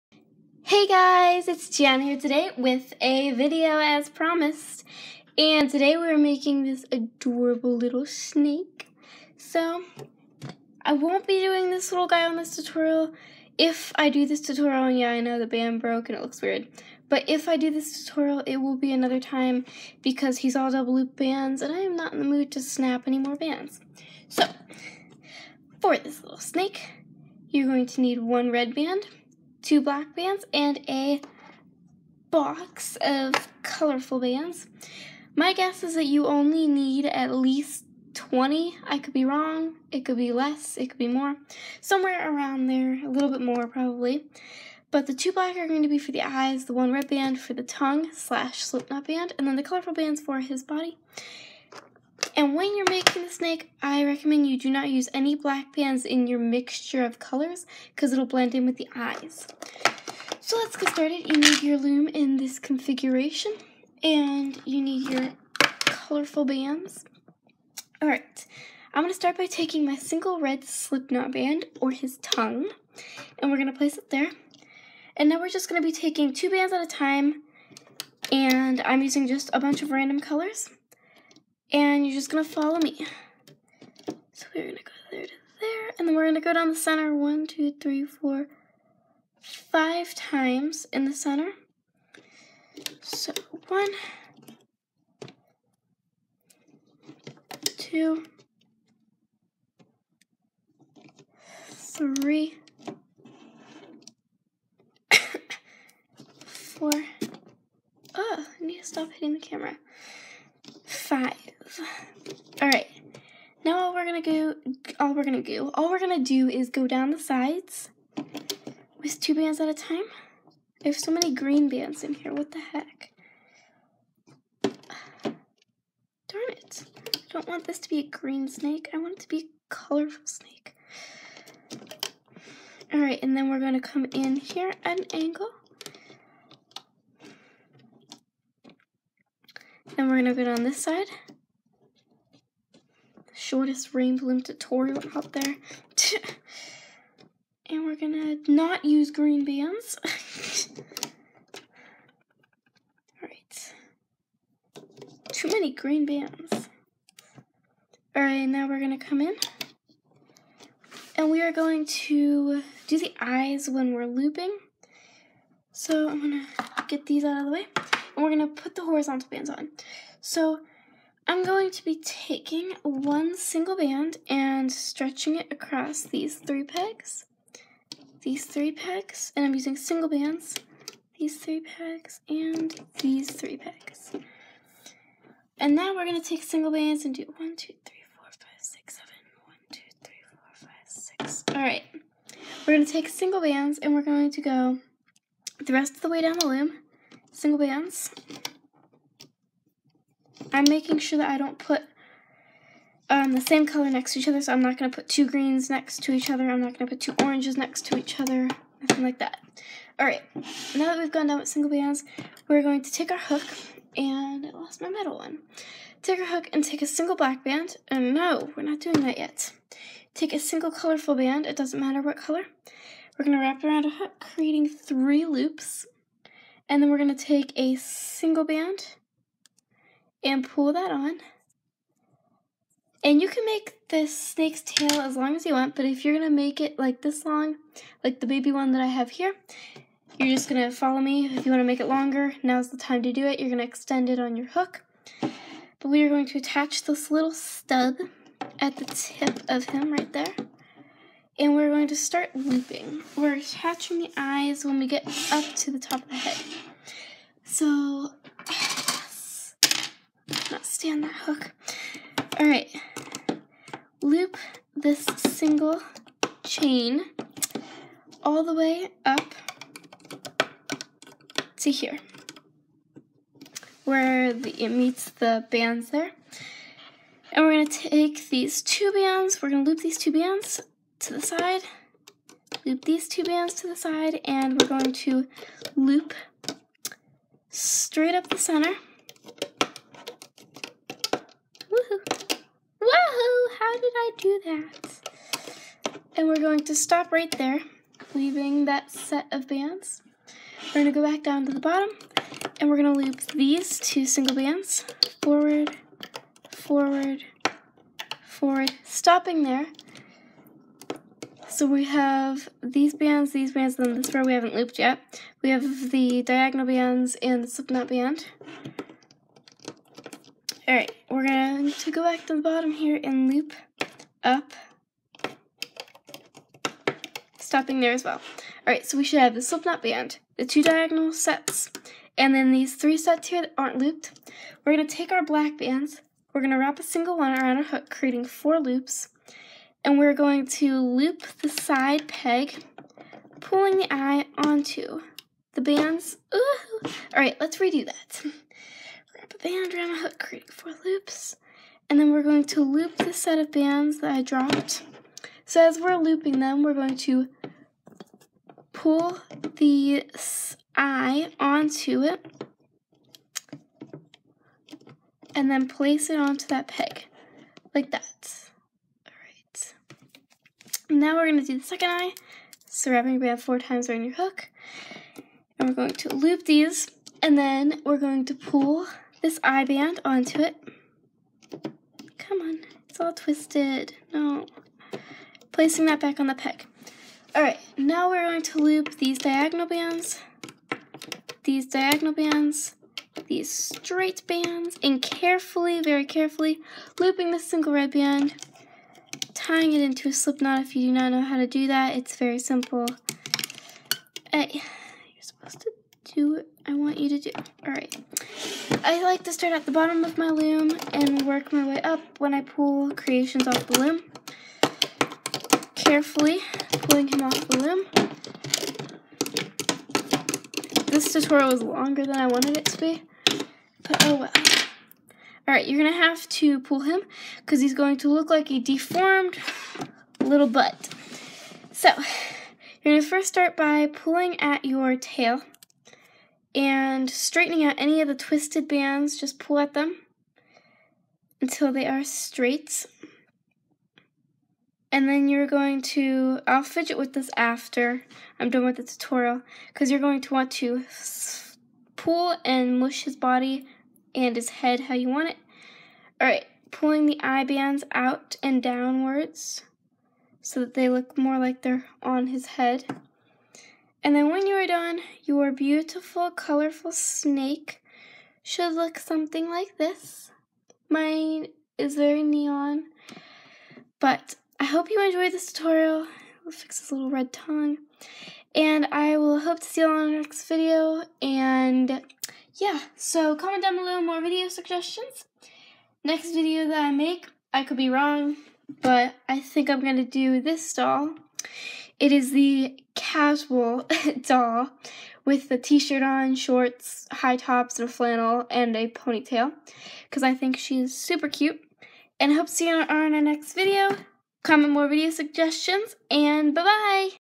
Rainbowloom tiny snake
Today I did the baby snake as promised 😀
Approx. band count
1 red band
2 black bands
And 46 random colorful bands
Hope you like him he is a very simple adorable snake 😍
-Gianna